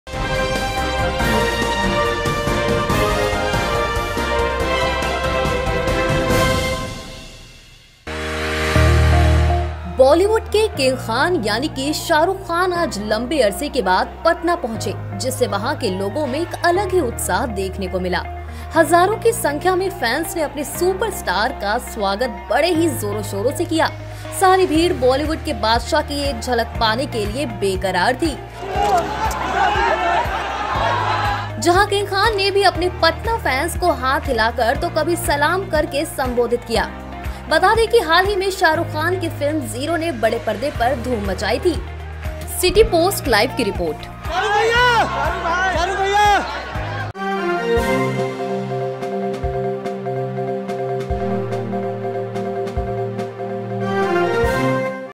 पटना आकर बिहारियों के दीवाने हुए शाहरुख
बॉलीवुड के के किंग खान यानि की शाहरुख खान आज लम्बे आरसे के बाद  पटना पहुंचे जिससे वहां के लोगो में एक अलग ही उत्साह देखने को मिला।